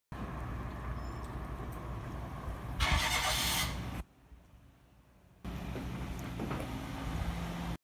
たちばなや食堂 2018年12月
『平成30年10月22日に貨物船が橋に衝突して通行止めと全島断水になり』

山口県周防大島町＝たちばなや食堂

中華そば＝600円

断水から安下庄の水道が使えるようになったのが昨日とのこと

風評被害で飲食店への来客がほぼ途絶えていたが

島民に元気を与える為にも休まず営業を続けていた。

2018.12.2